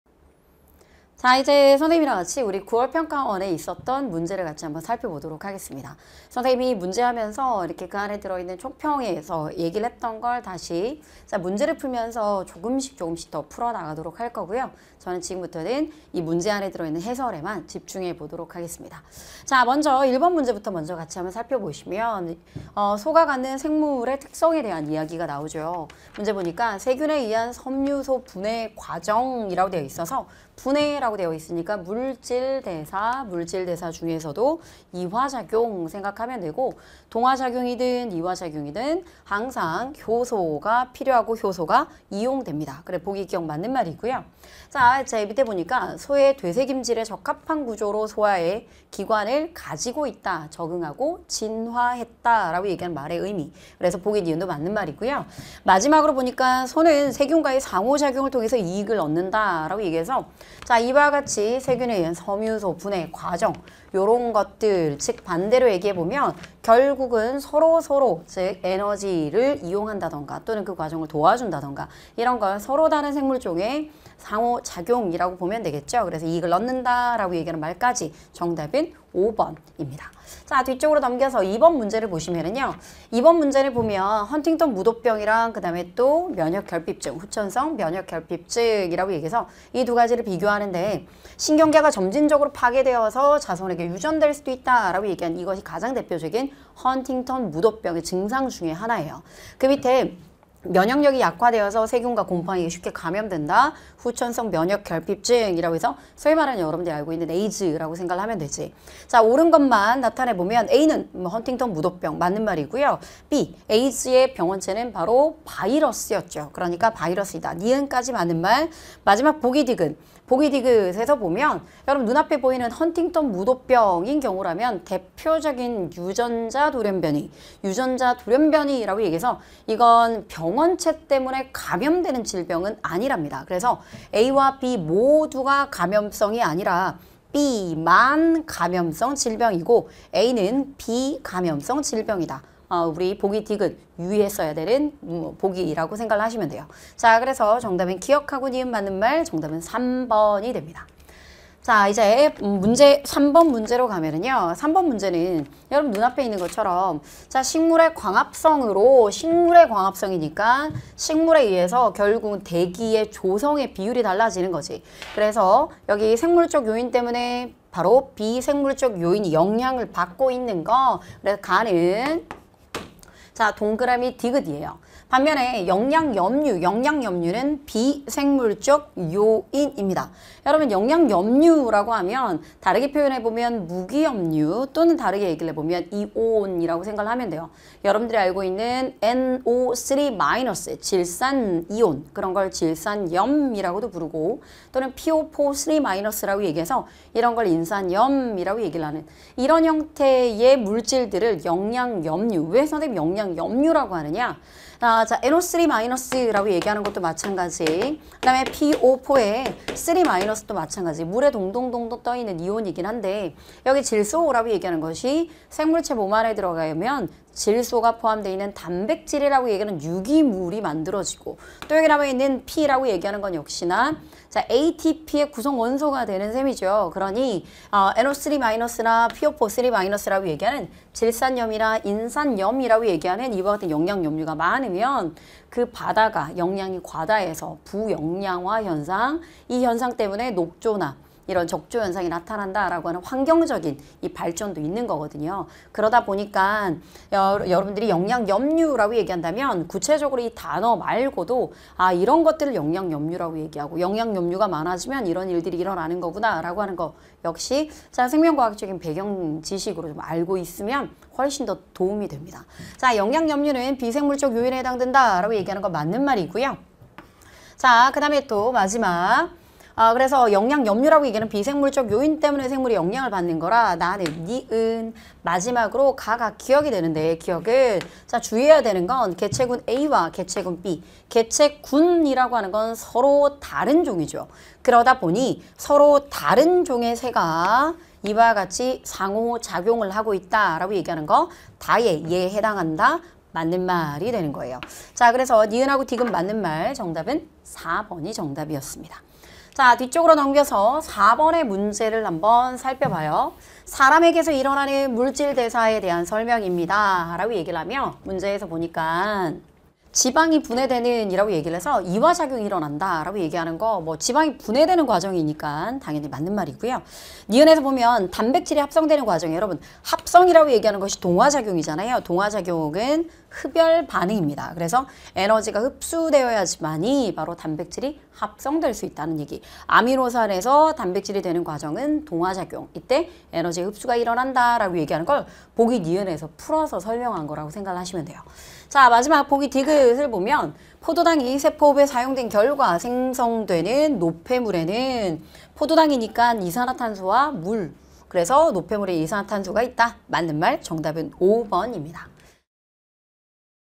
[메가스터디] 생명과학 박지향쌤 - 2023학년도 9월 모평 고3 생명과학l 해설 강의
생명과학 박지향 선생님의 2023학년도 9월 모평 고3 생명과학l 해설 강의 영상입니다.

- 생명과학 박지향 선생님을 더 알고 싶다면?